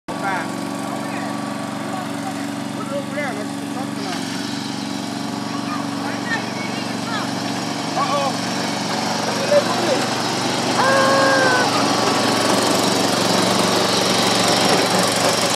Sandra attempts to run over sons you with lawn mower
Sandra and son almost run over his toy with riding lawn mower lmao